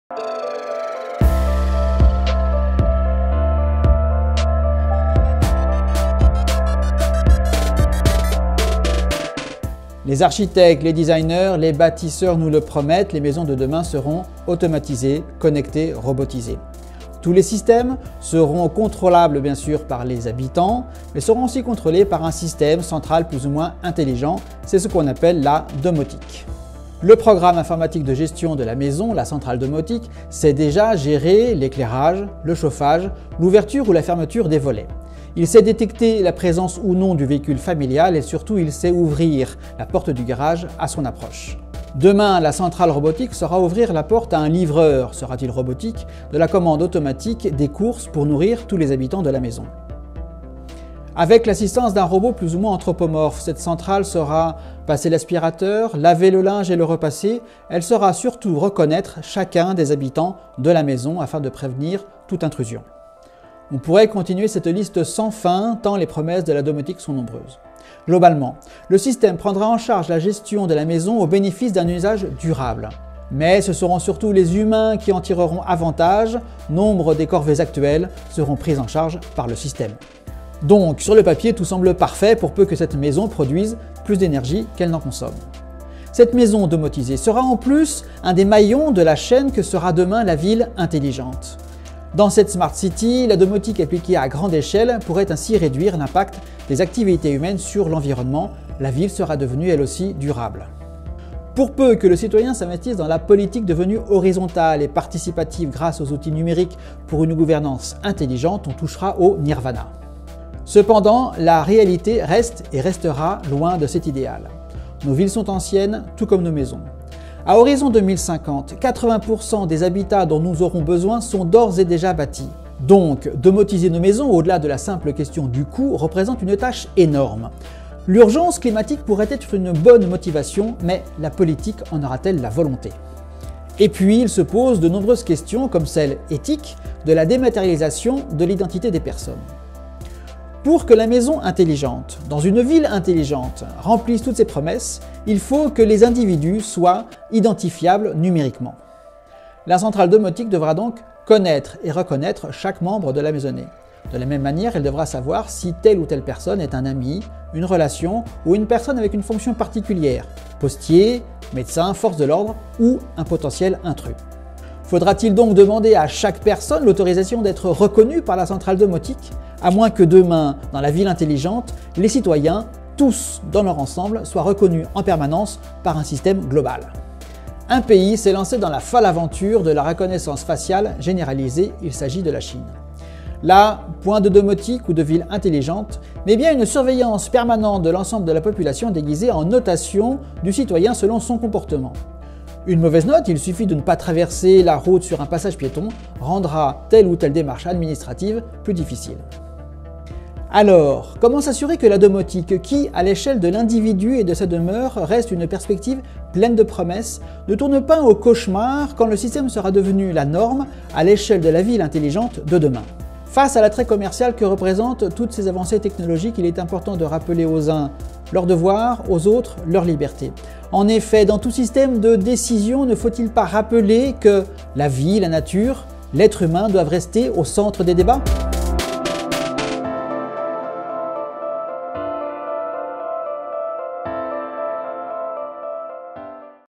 Ce que la DOMOTIQUE nous dit sur demain | Analyse prospectiviste du quotidien
Deux ou trois choses que « LA DOMOTIQUE », ou ce qu'on en imagine, auraient à nous dire sur demain…
« Dis-moi quel film tu regardes, je te dirai quel avenir tu te prépares » pourrait tout aussi bien dire le prospectiviste Olivier Parent…